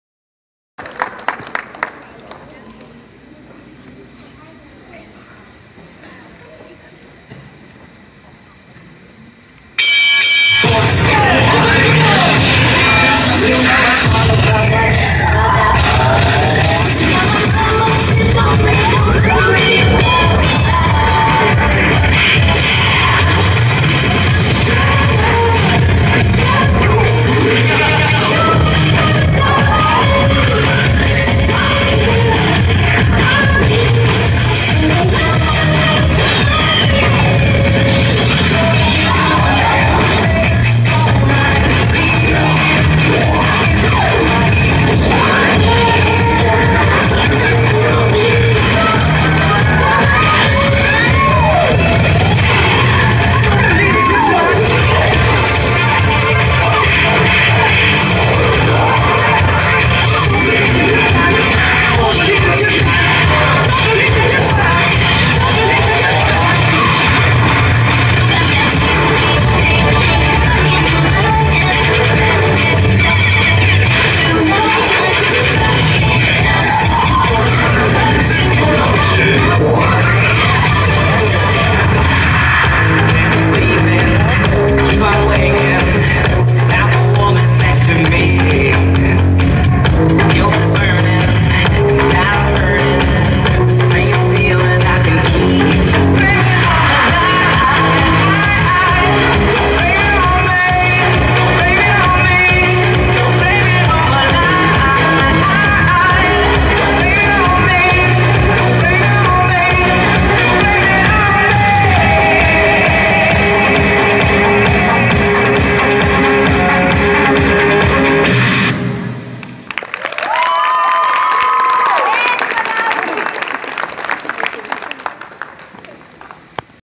NSMS Fillies Dance Team Pom Routine
Here is officer Ashley Hill along with her North Shore Middle School Fillies Dance Team performing their pom routine choreographed by the Dance Intensive Company for their 2016 spring show!